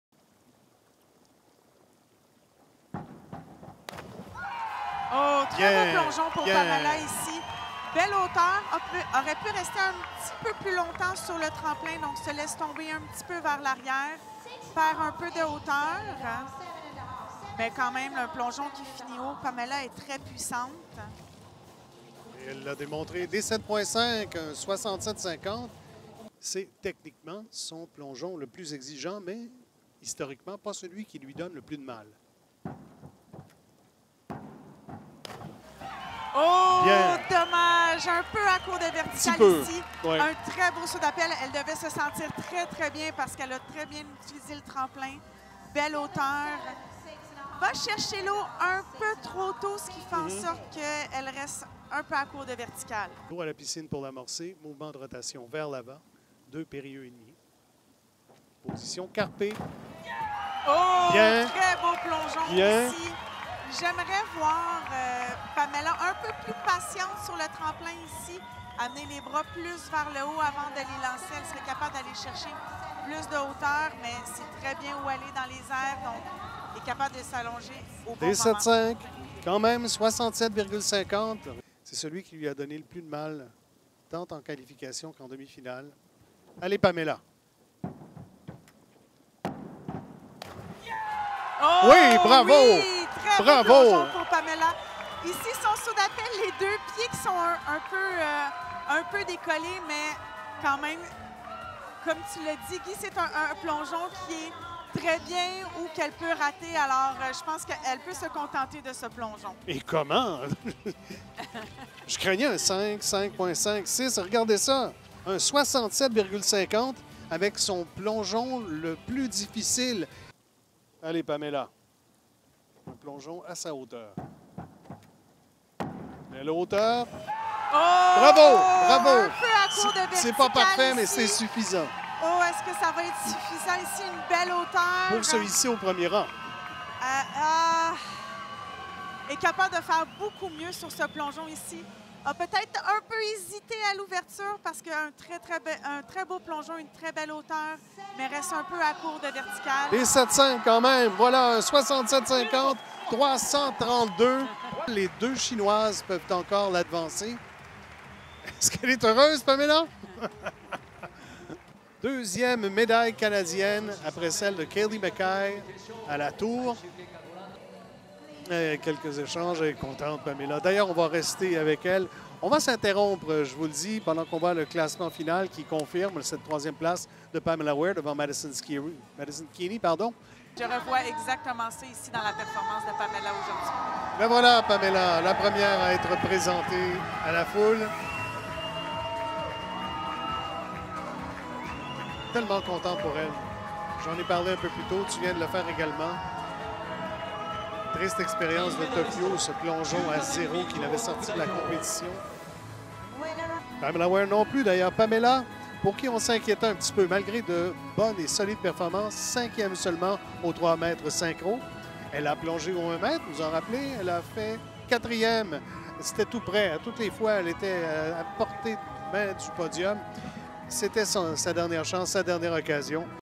Pamela Ware remporte le bronze au tremplin de 3 m
Pamela Ware a remporté la médaille de bronze au tremplin de 3 m, la deuxième médaille canadienne des mondiaux de Fukuoka. (Images : World Aquatics)